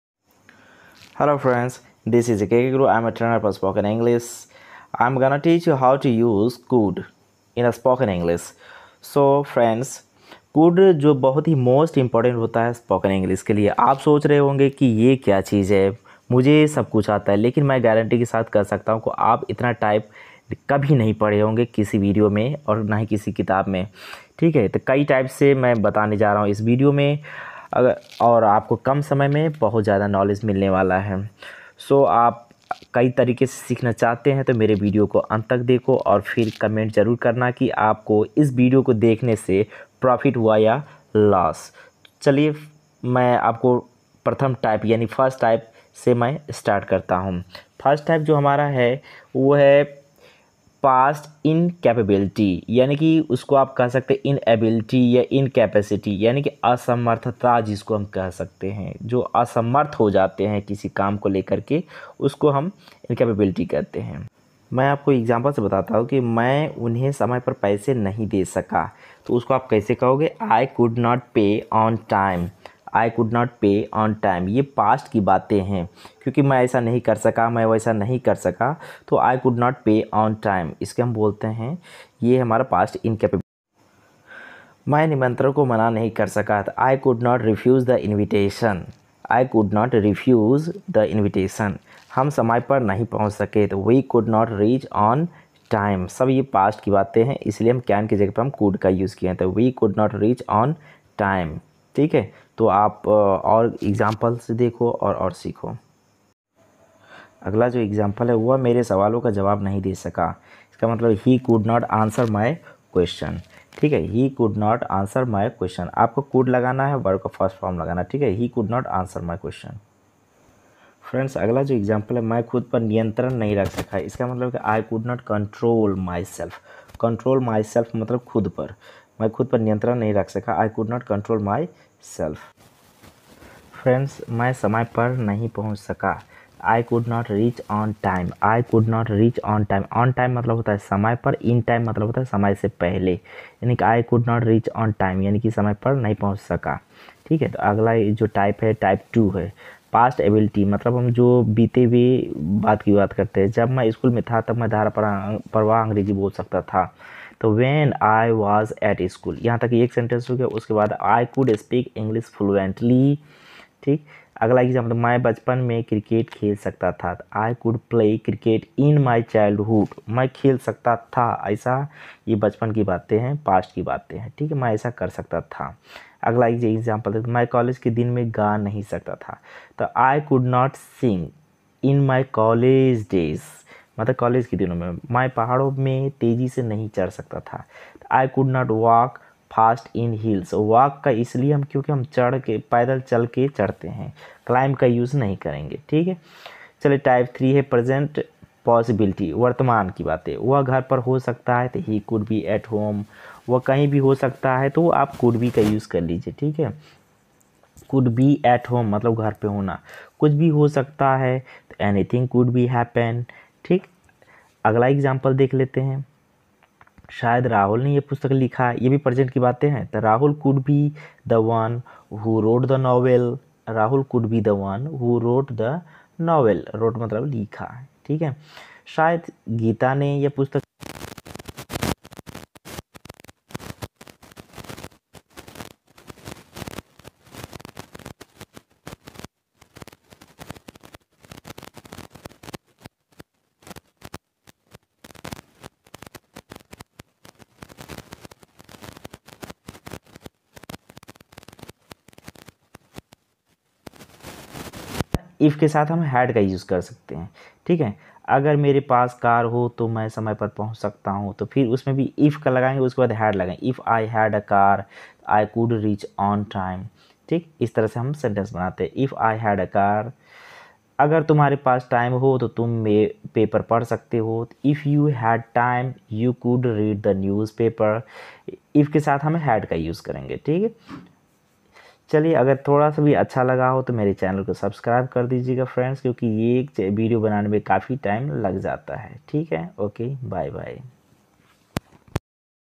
Use of Could( could का प्रयोग) by K.K. kkjaunpuriya ।। KK Jaunpuriya अंग्रेजी बोलना कैसे सीखें
Could का प्रयोग आप नही जानते हो तो अंग्रेजी बोलना नही आएगा । could का कई प्रकार हमने आपको बताया है उसको सीखो ।